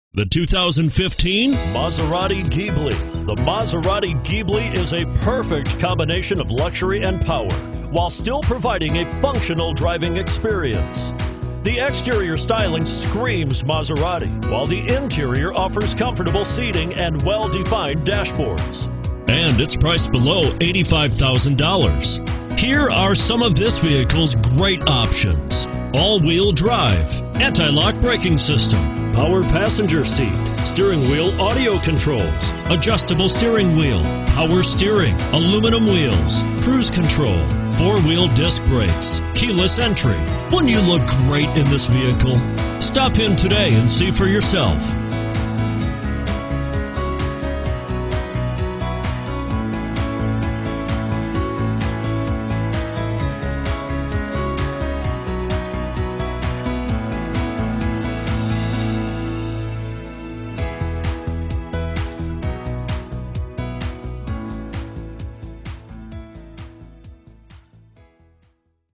2015 MASERATI GHIBLI S Cincinnati, OH MN-F1132574
2015 MASERATI GHIBLI S Cincinnati, OH
Stock# MN-F1132574

Beechmont Motors
4113 Plainville Rd

Don't miss this 2015 Maserati Ghibli S. It's equipped with Automatic transmission and features a Nero exterior. With 0 miles, you'll want to take this car home. Make a great choice today.